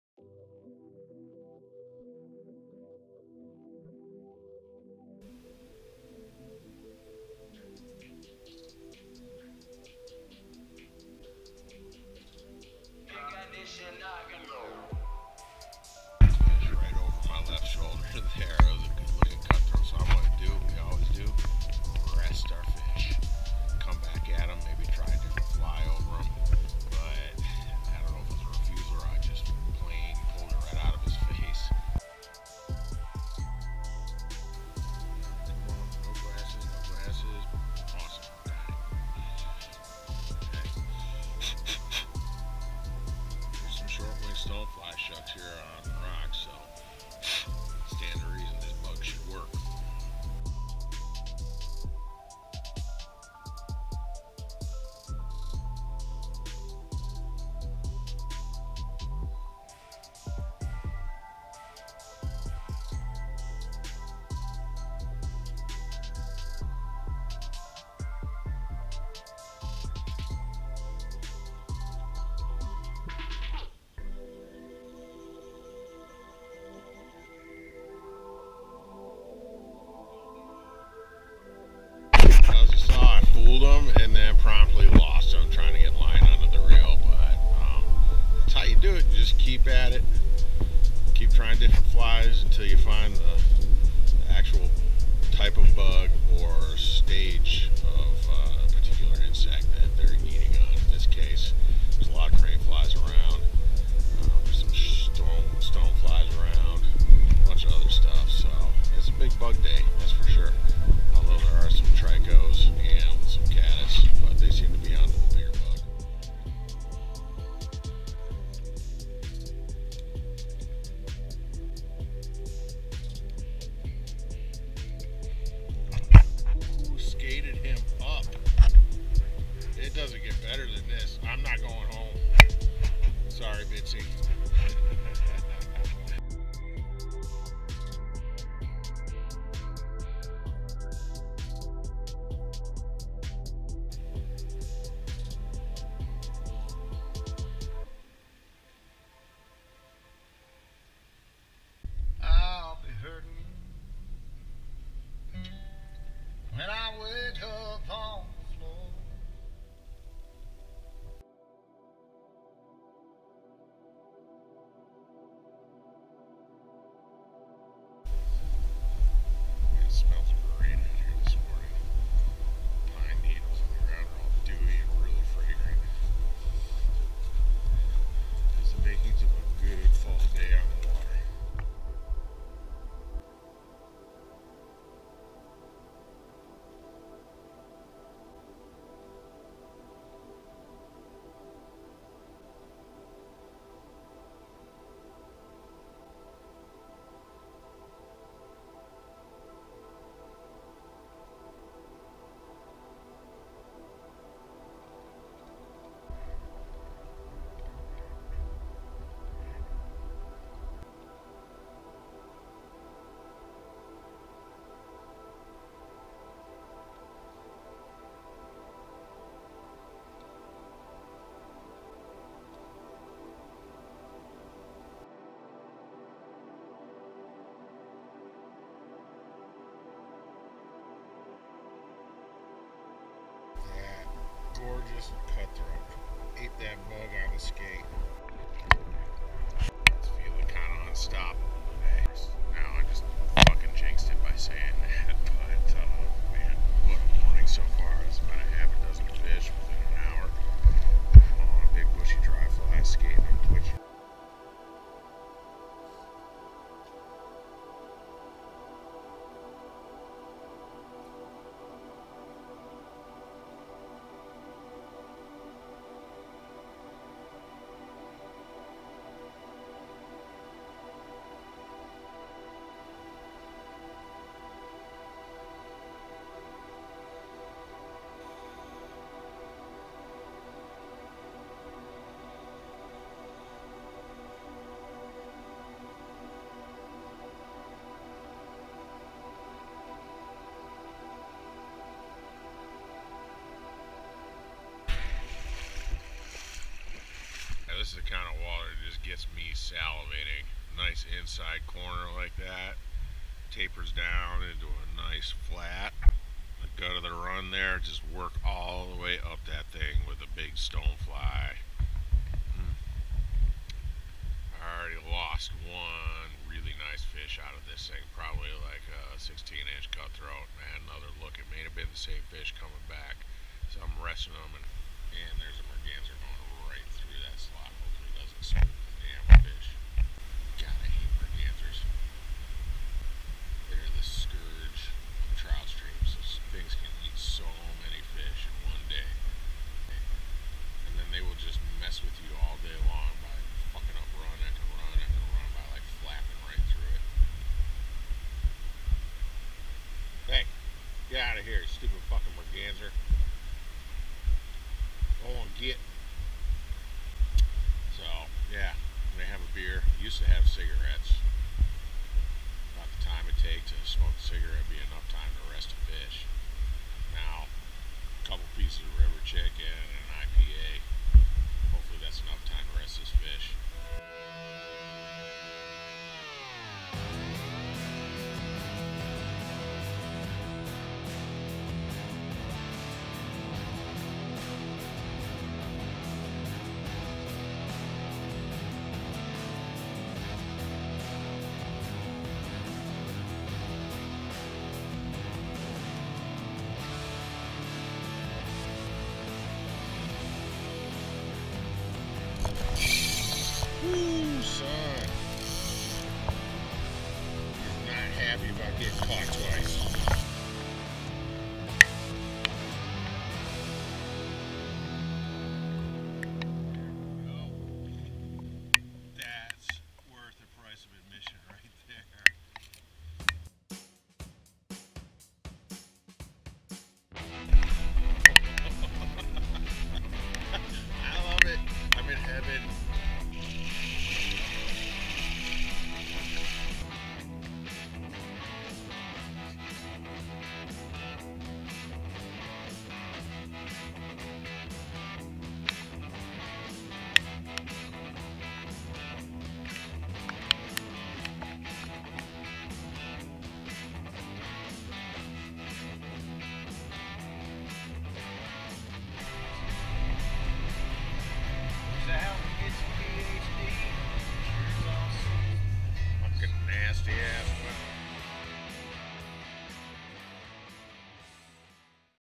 Fall dry fly fishing tactics in Washington
Providing some of the stuff that works for me after fishing in this state for twenty years.